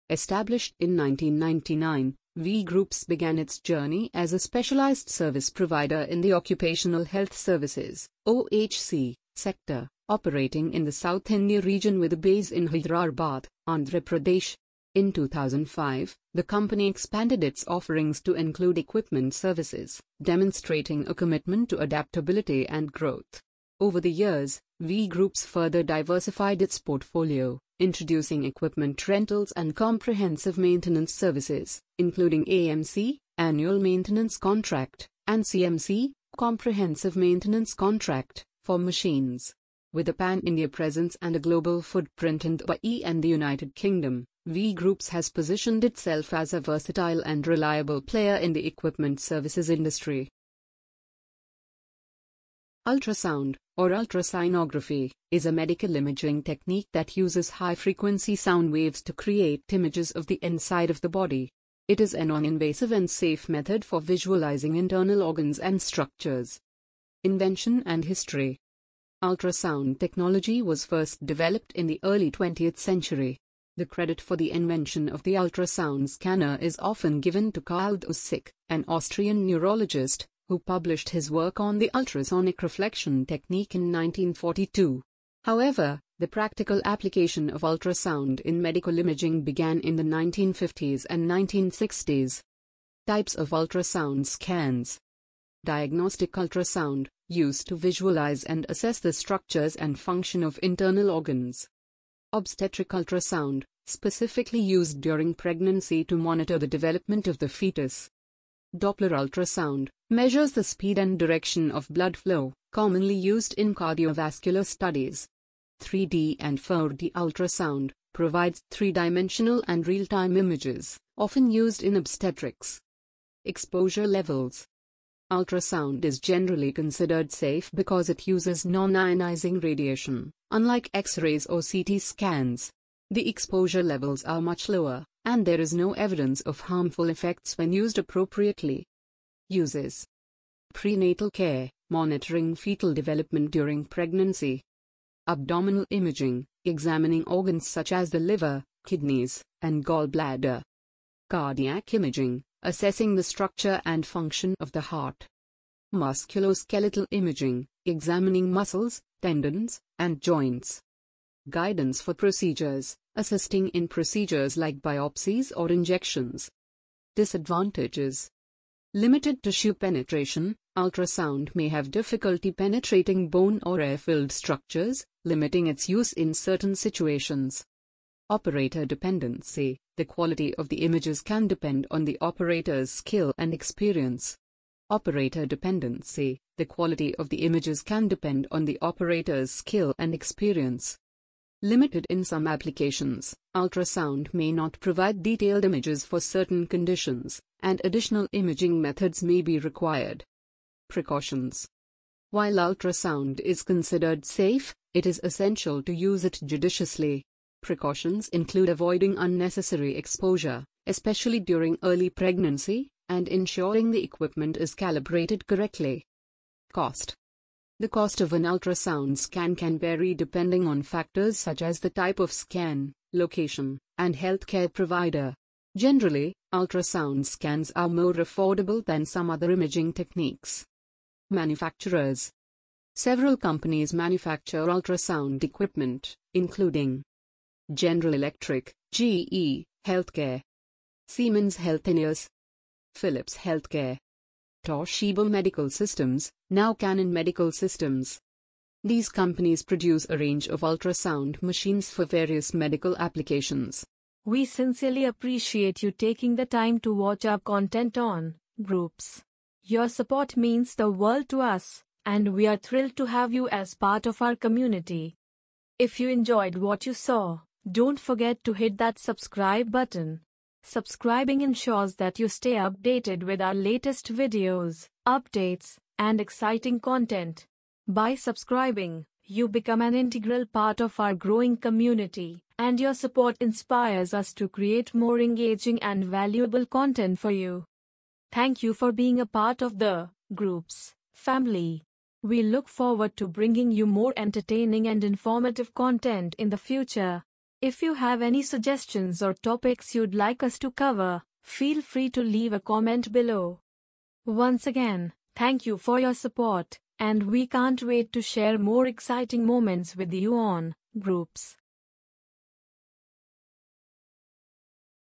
Wonders of Ultrasound:
Dive into the fascinating world of ultrasound technology with our comprehensive exploration! Learn about its intriguing history, from Karl Dussik's breakthrough to today's advanced medical imaging. Discover the different types of ultrasound scans and their diverse applications, from prenatal care to cardiac imaging. We'll also delve into the safety aspects, benefits, and limitations of ultrasound, providing valuable insights for both healthcare professionals and curious minds. Join us on this journey through sound waves and medical marvels! 🎥✨

🚀 Topics Covered:

History and Invention of Ultrasound
Types of Ultrasound Scans: Obstetric, Diagnostic, Doppler, and more!
Exposure Levels and Safety
Real-World Uses: Prenatal Care, Cardiac Imaging, and Beyond
Advantages and Disadvantages of Ultrasound Technology